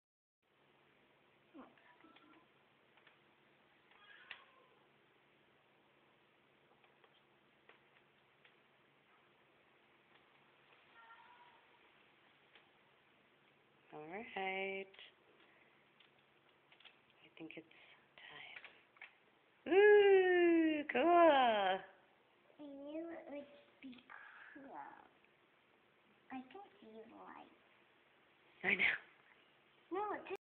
Lite Brite
Found this oldie but goodie at a garage sale. Finally fired it up at the end of a tutoring session today. Fun!